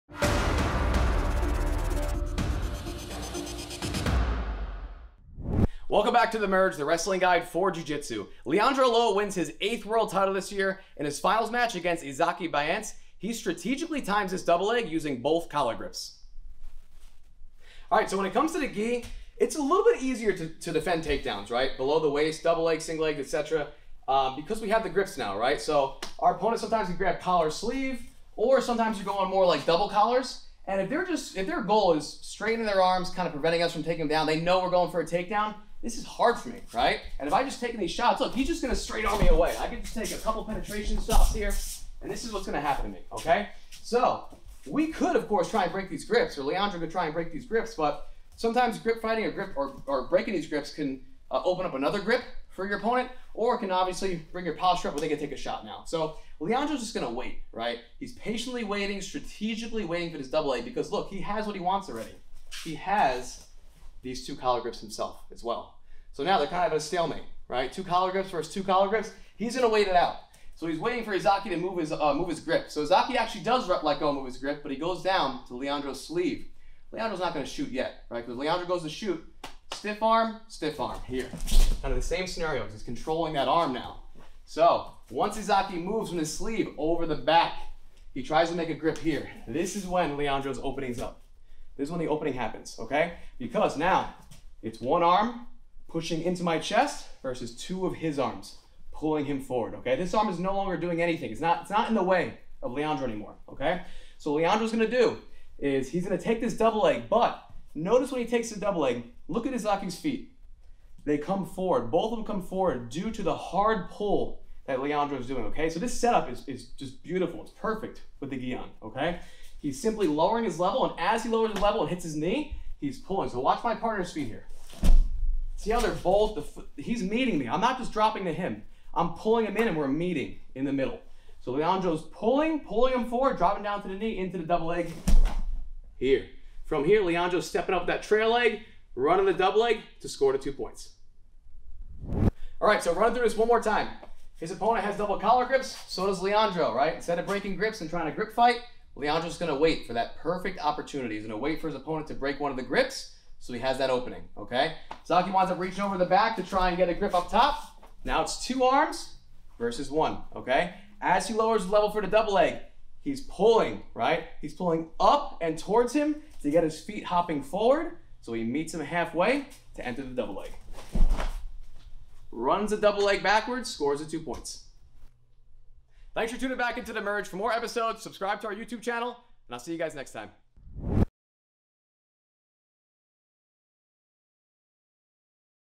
The Merge Ep. 96 - Leandro Lo's Double Leg - Collar Grip Setup
2022 IBJJF World Championships, Leandro Lo makes history becoming the fourth male athlete to win eight world titles. In his finals match against Isaque Bahiense, he hits his commonly seen double leg off double collar grips. Michael highlights Lo's patient strategy with the double leg, how the gi grips changed his approach, and his effective setup.

0:00 - Intro
0:17 - Leandro's Double Leg
2:56 - Outro